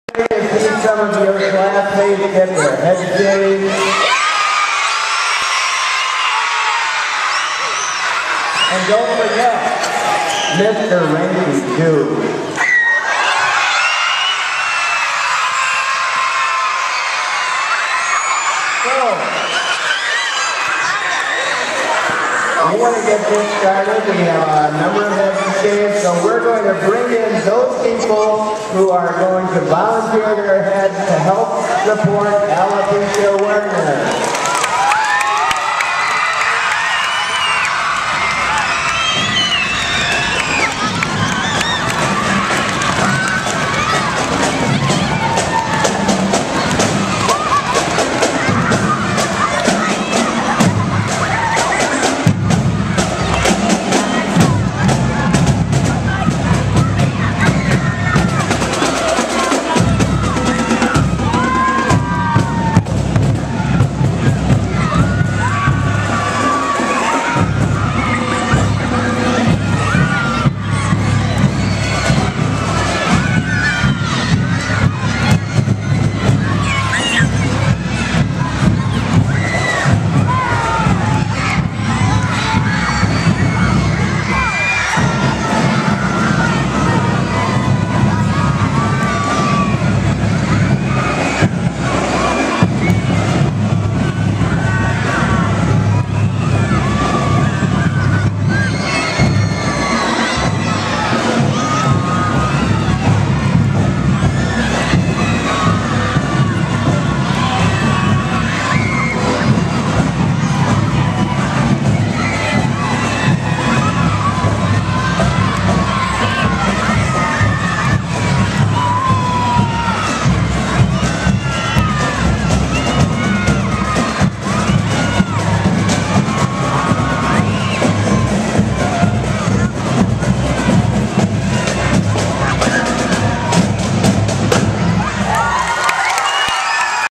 Drumline Entrance - Alopecia Awareness Assembly 2017
As part of our "Reagan Gives Back" program our school raised over $1,500 in support of National Alopecia Areata Awareness Month and our classmate Ava.  Ten brave students raised money to help the cause and have their heads shaved at an assembly in front of all their school mates.  This is their grand entrance.